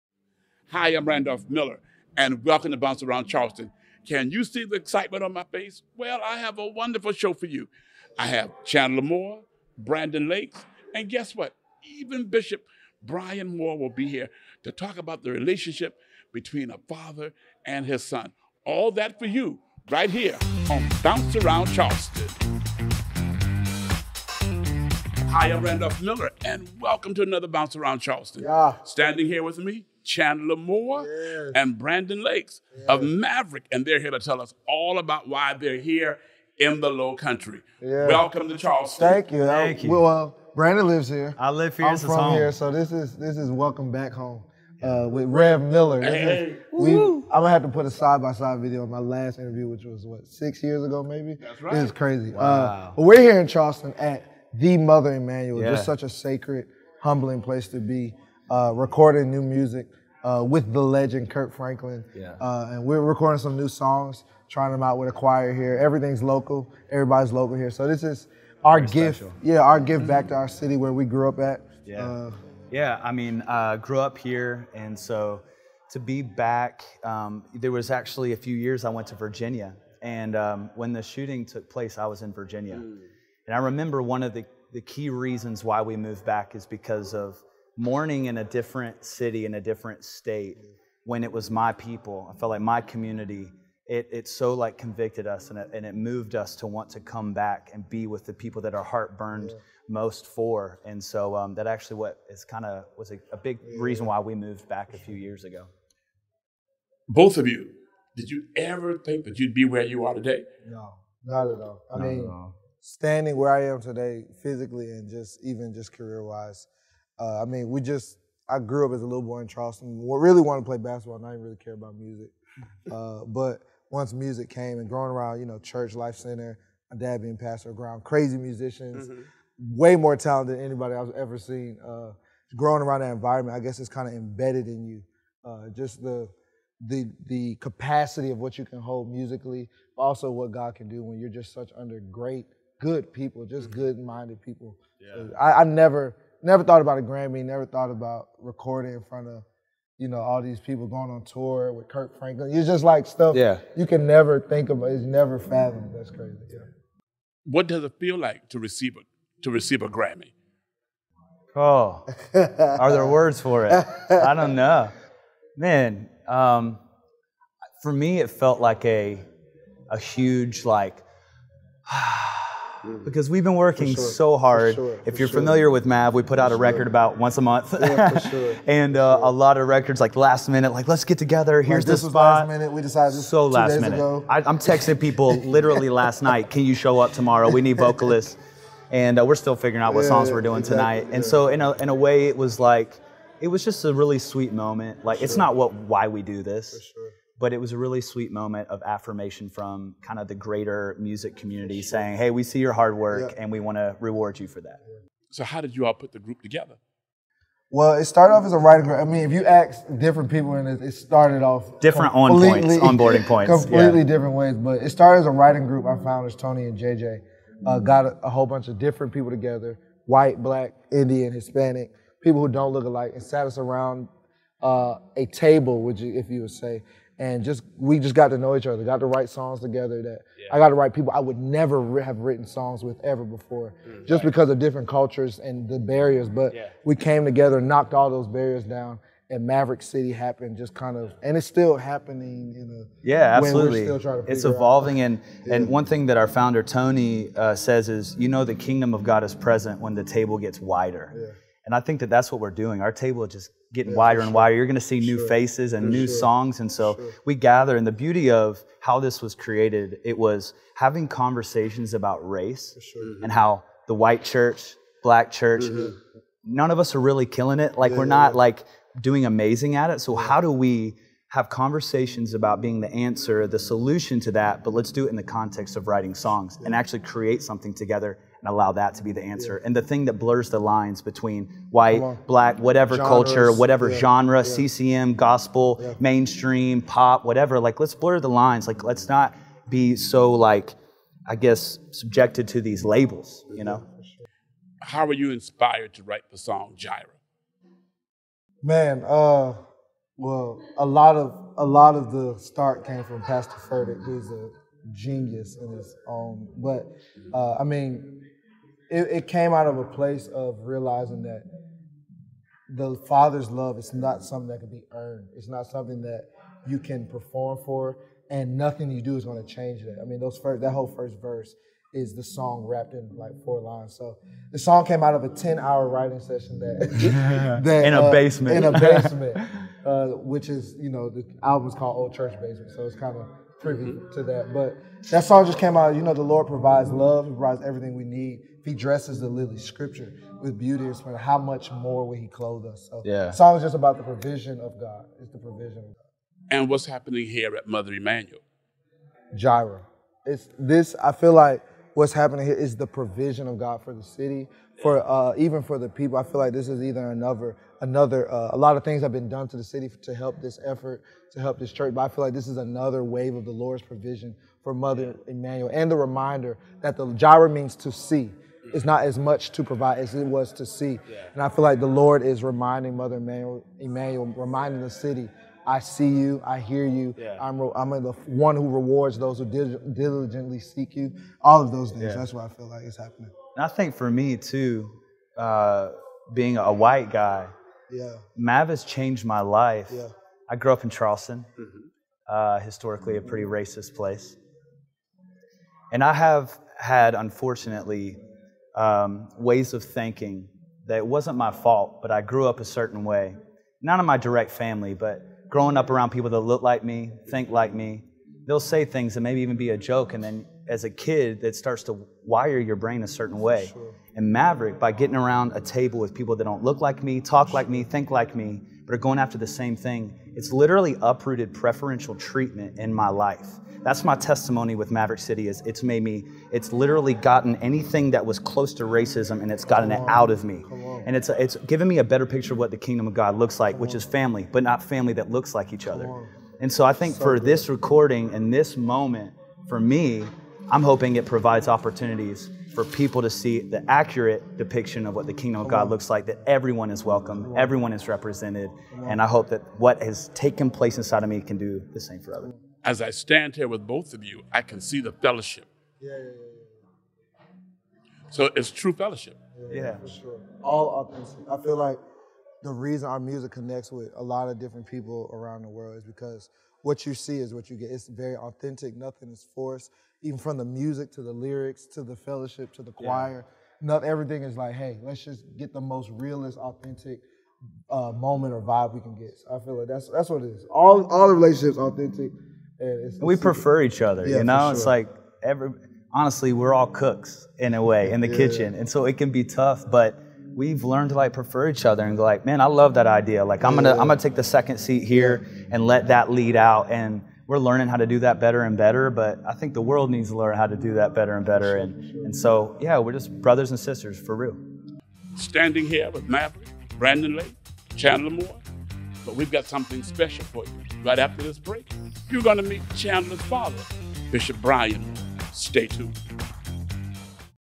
Rev. Miller talks to MAVERICK CITY - Chandler Moore and Brandon Lake at Emanuel AME
Chandler Moore and Brandon Lake of Maverick City Music were in Charleston to record some music at Emanuel AME Church.

Rev. Miller caught up with them to talk about music, faith, and city where they grew up.